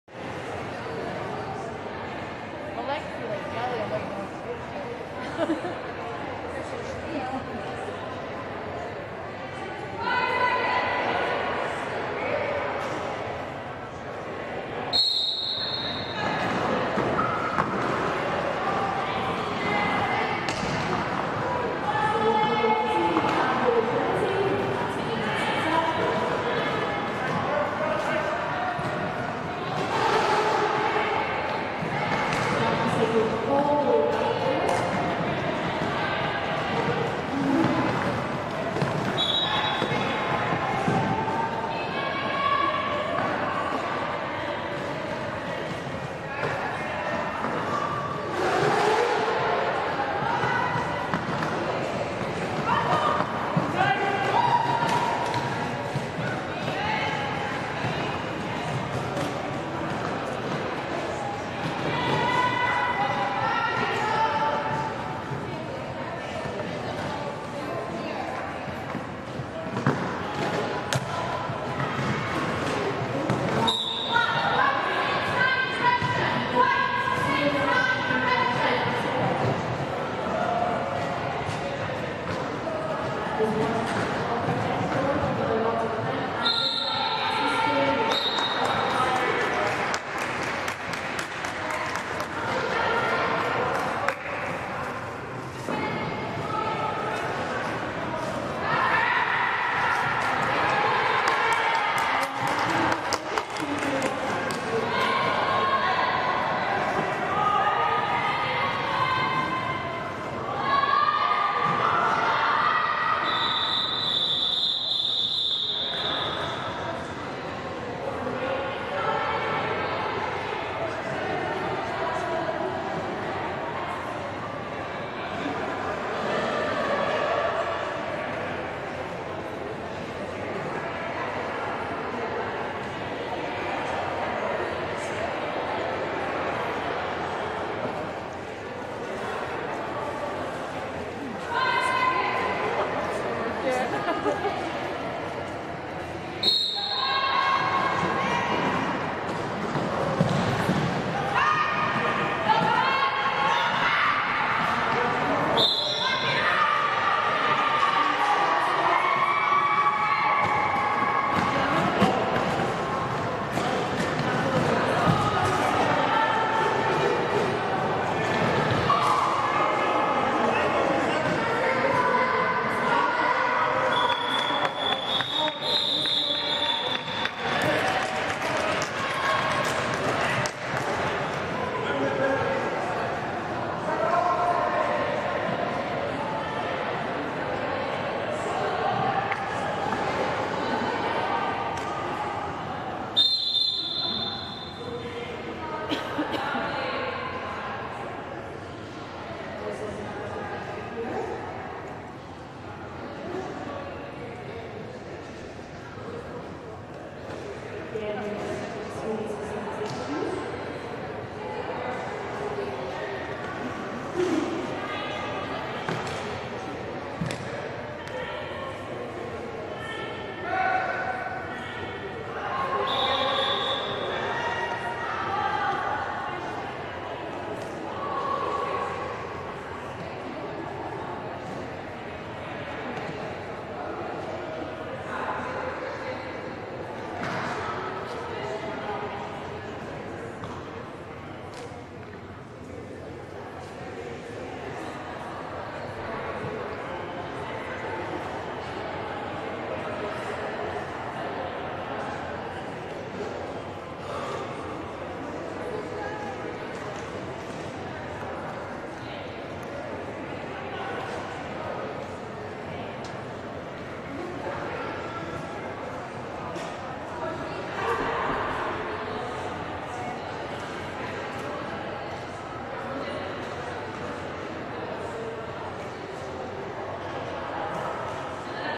Mean City v "Team White" - Period 1 Part 4 - 10 Feb 2024
Mean City Roller Derby host a double-header in Grangemouth Sports Complex.
This is the first game, Mean City versus a pickup team.
This is the fourth part of Period 1.